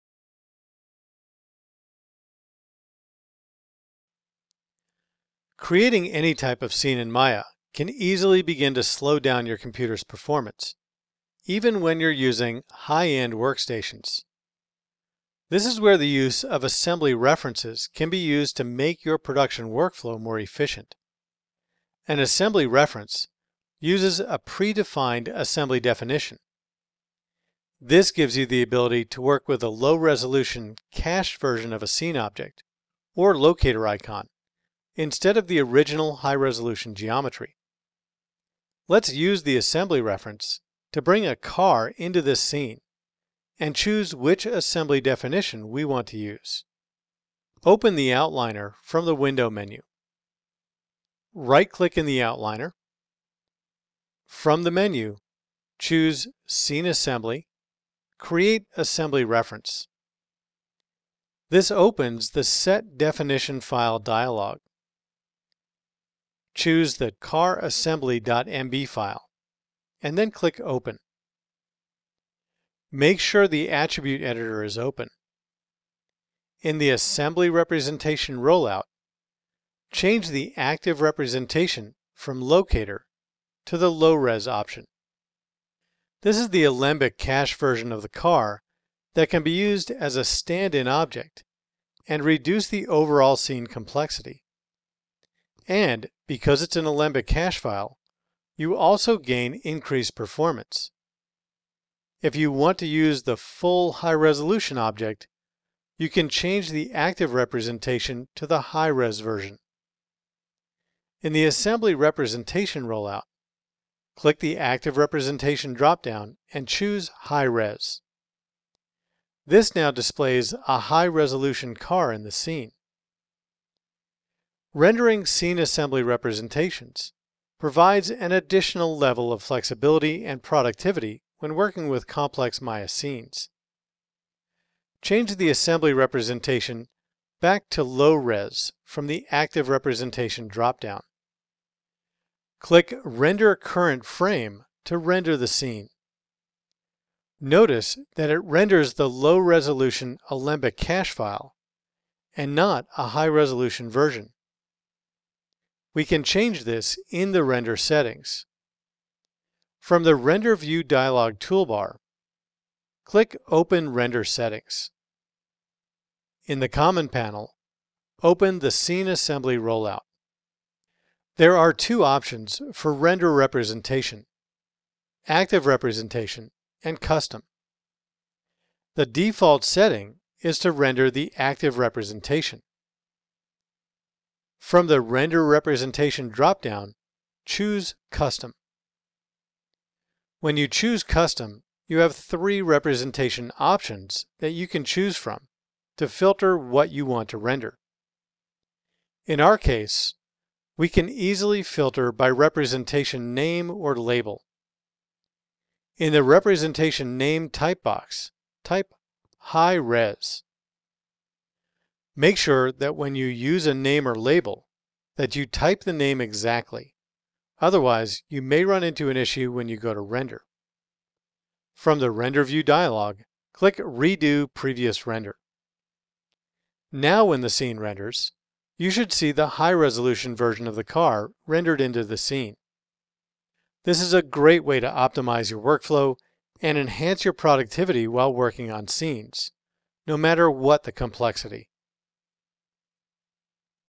Adding an Assembly Reference
Learn how to incorporate an Alembic file to provide for data exchange in Autodesk Maya 2015.